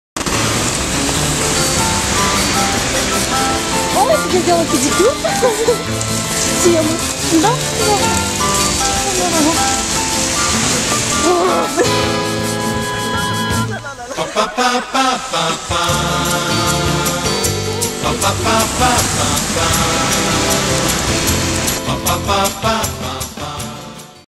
Ноги-1.wmv
Кто еще бегал недавно босиком под дождем? ) Первая серия сериала ноги. Продолжение - скоро!